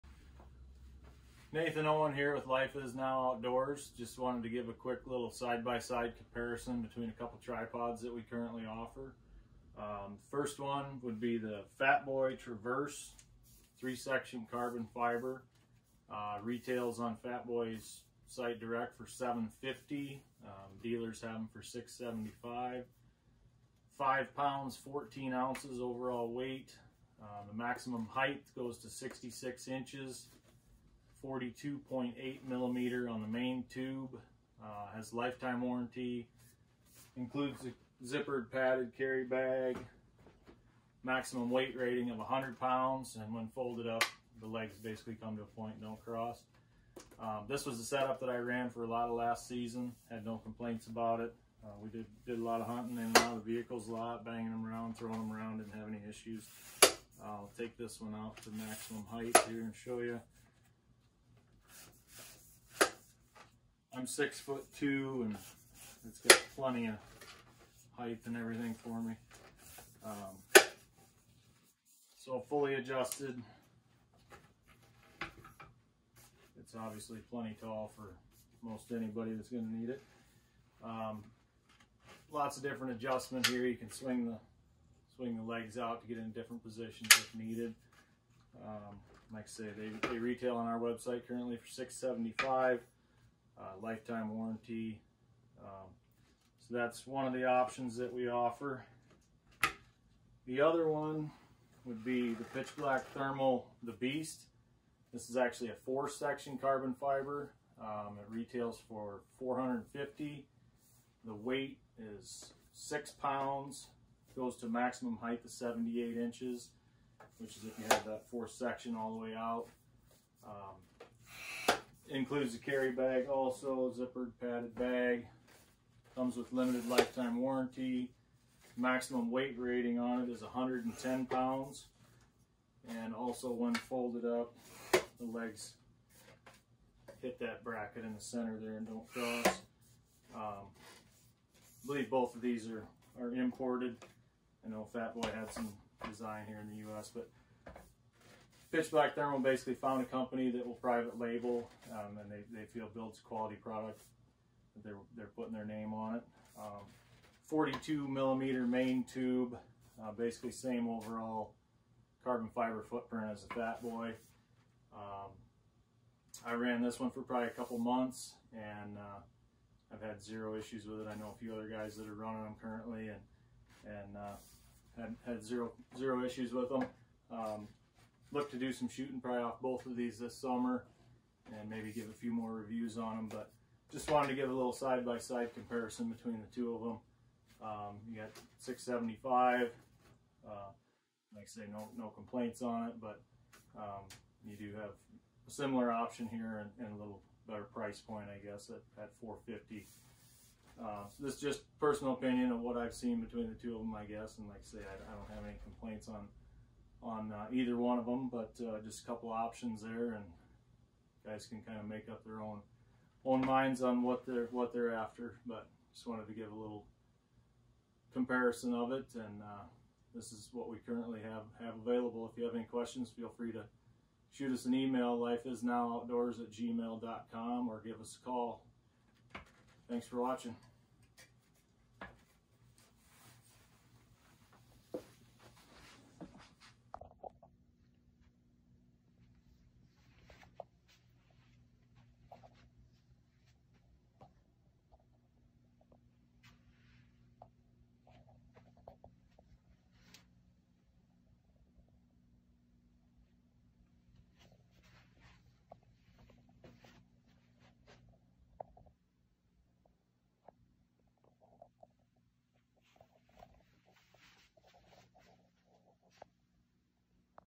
FATBOY Traverse Tripod vs Pitch Black Thermal The Beast Tripod Comparison
Side by side spec comparison of the FATBOY Traverse tripod vs The Beast from Pitch Black Thermal.